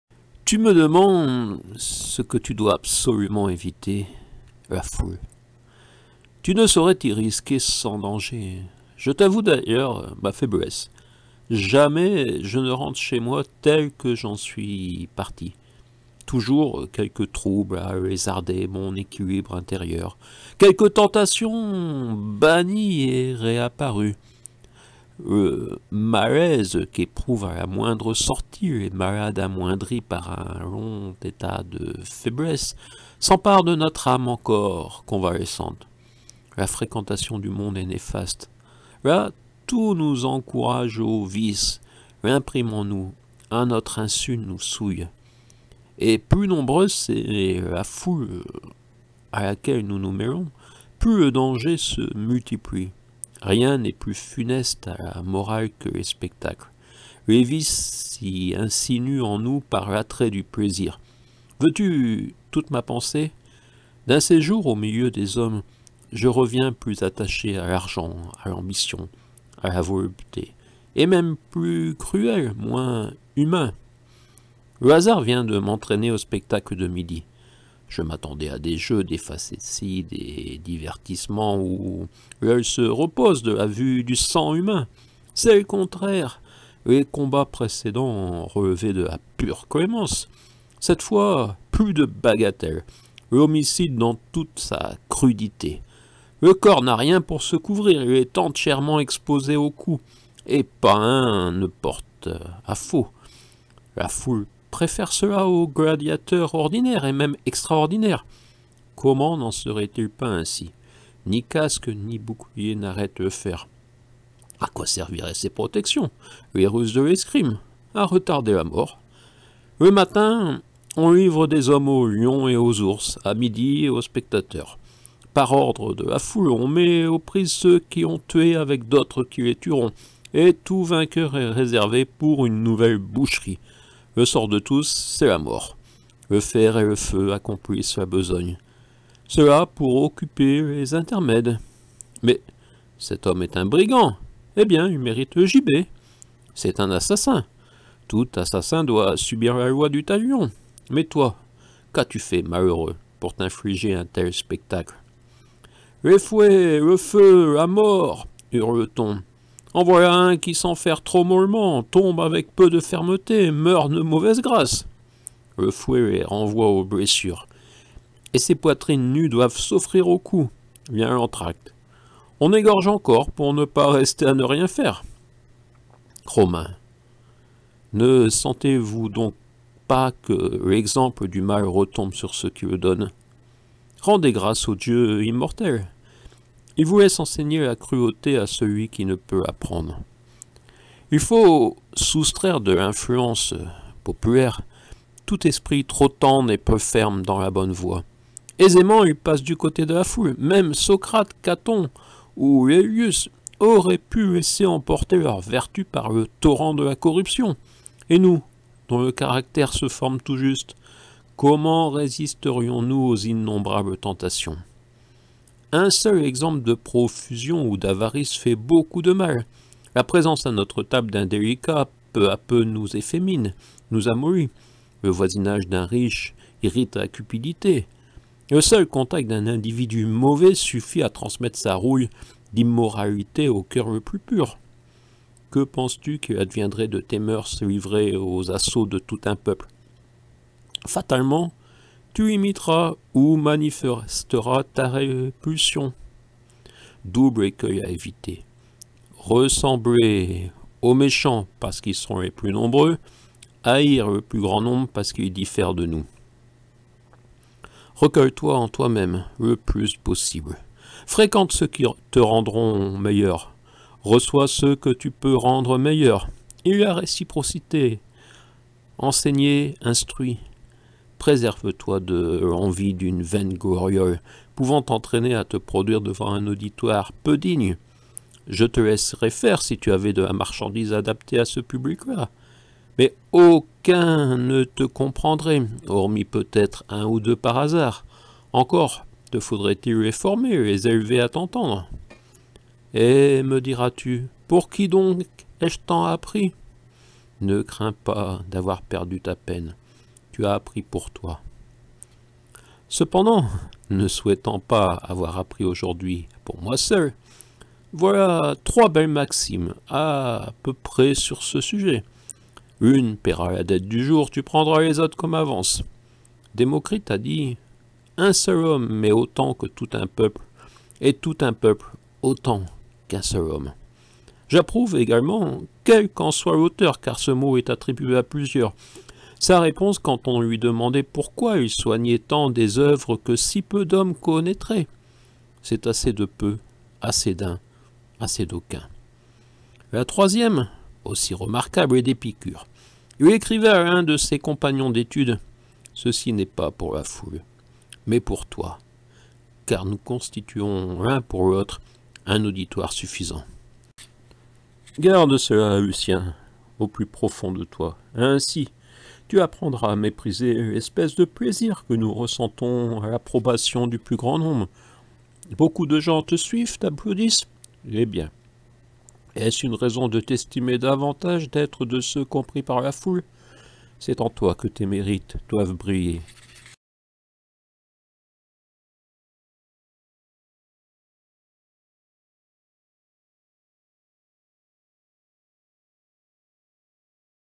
Sénèque Lettres 7 à Lucilius réécriture 2018 Fuir la foule Protéger esprits des mauvaises influences
Sénèque Lettres à Lucilius Lettre 7 : Réécriture 2018
Fuir la foule, les spectacles cruels, protéger les esprits des mauvaises influences

c'est gratuit, c’est rapide...